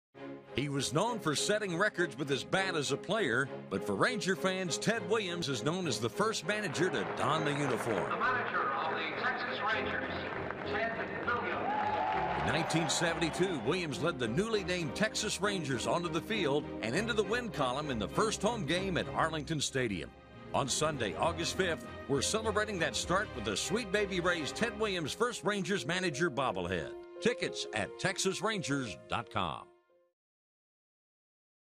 Texas Rangers Hall of Fame honors Ted Williams
The Texas Rangers are capping off their Hall of Fame weekend with the Sweet Baby Ray's Ted Williams First Rangers Manager bobblehead